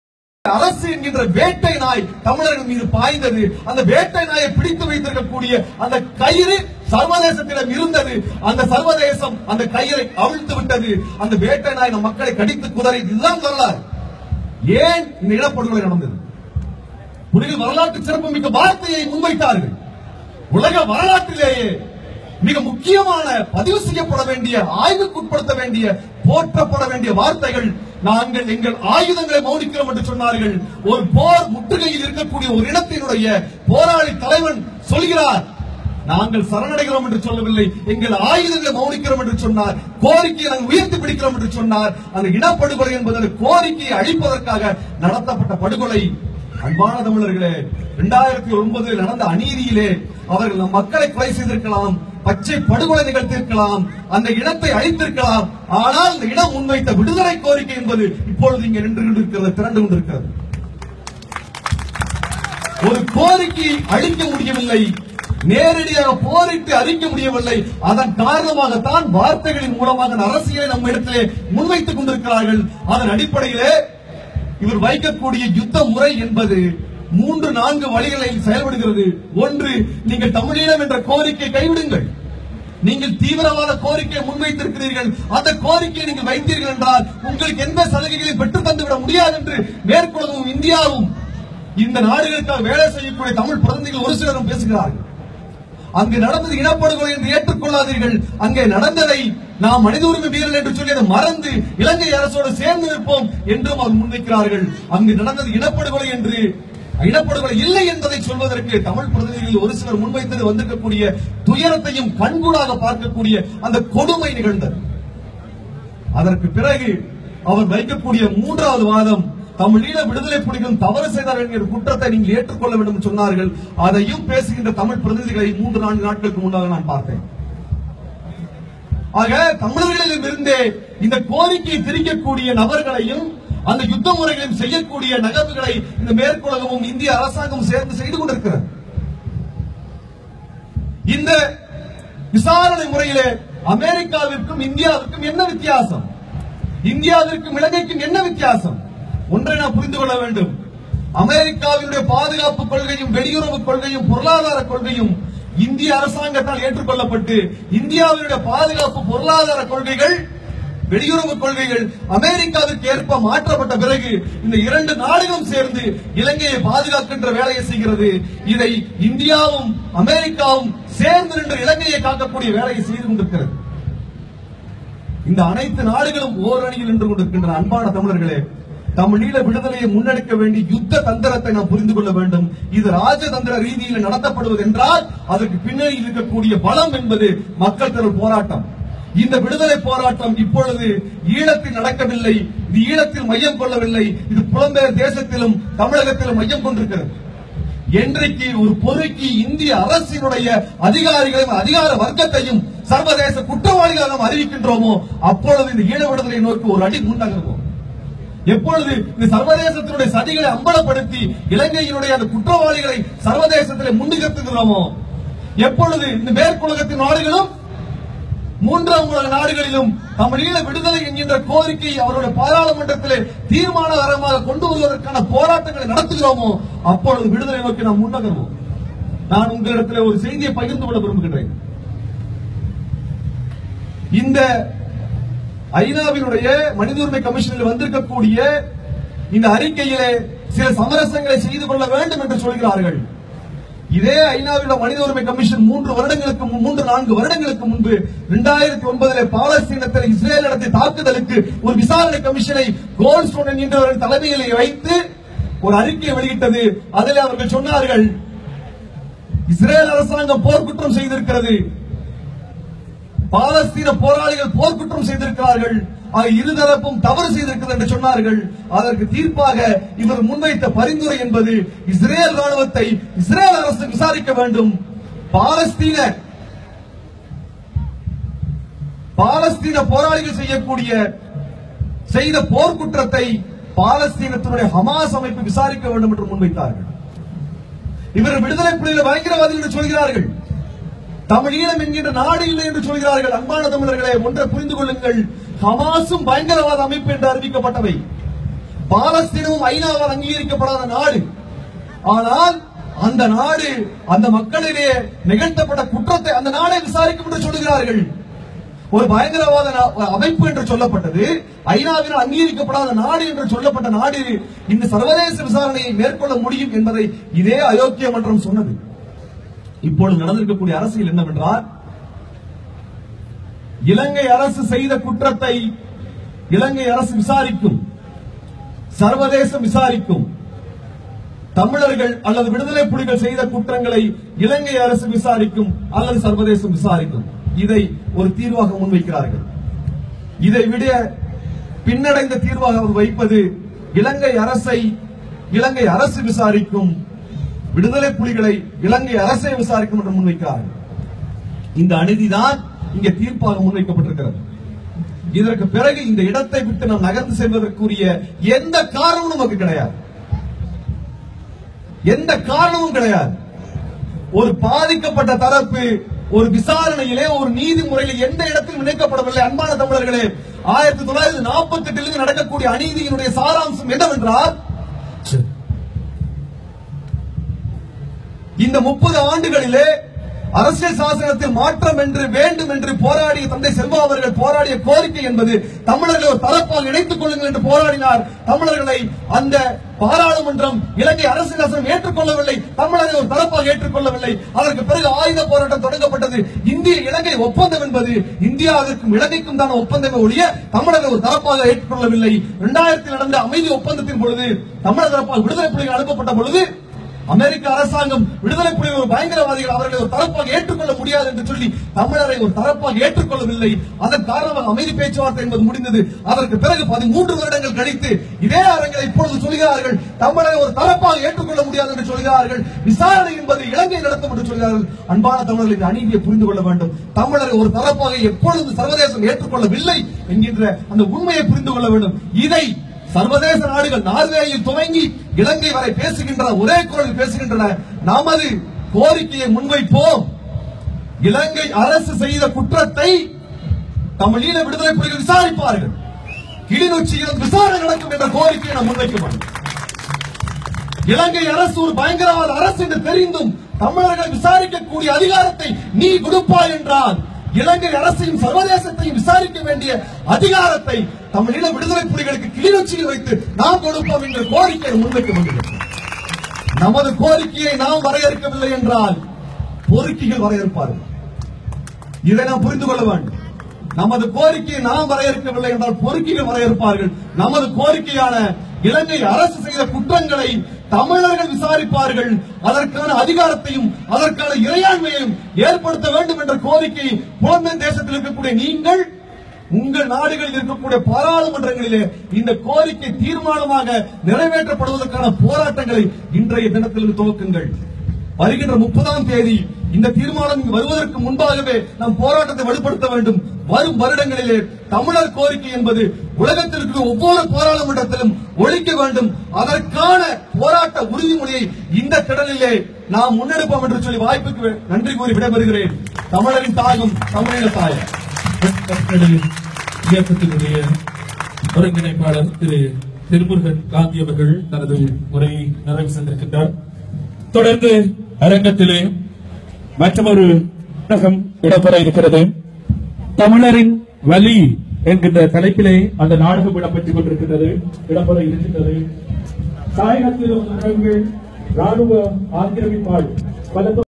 21.09.2015 அன்று புலம்பெயர்ந்த தமிழர்கள் மத்தியில் Thirumurugan Gandhi. பகுதி-2,part first linkbelow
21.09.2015 அன்று புலம்பெயர்ந்த தமிழர்கள் மத்தியில் Thirumurugan Gandhi.
பகுதி-2